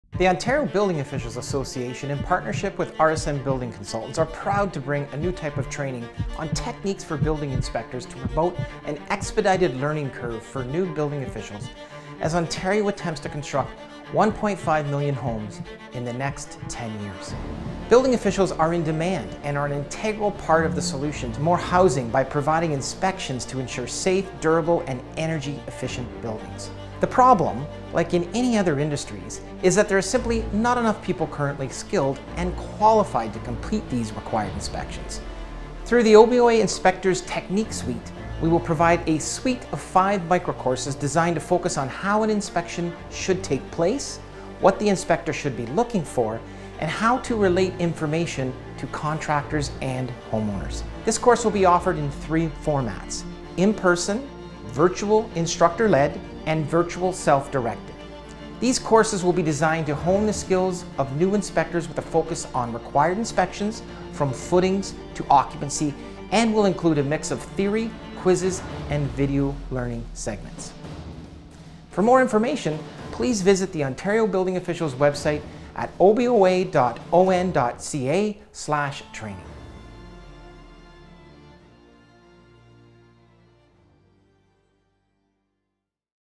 Ontario Building Officials Association Inspectors Technique Suite
The Ontario Building Officials Association in partnership with RSM Building Consultants are proud to be bringing a new type of training on techniques for Building Inspections to promote an expedited learning curve for new Building Officials as Ontario Attempts to construct 1.5 million new dwellings units in the next 10 years.

Building Officials are in demand and are an integral part of the solution to more housing by providing inspections to ensure safe, durable and energy efficient buildings.

 The problem, like in many other industries, is that there are simply not enough people currently skilled and qualified to complete these required inspections.  Through the OBOA Inspectors’ Technique Suite we will provide a suite of five micro courses designed to focus on how an inspection should take place, what the inspector should be looking for, and how and how to relate information to contractors and homeowners.

 The course will be offered in three formats:  In person, virtual Instructor-led and virtual self-directed.

These courses will be designed to hone the skills of new inspectors with a focus on required inspections from footings to occupancy and will include a mix of theory, quizzes, and video learning segments.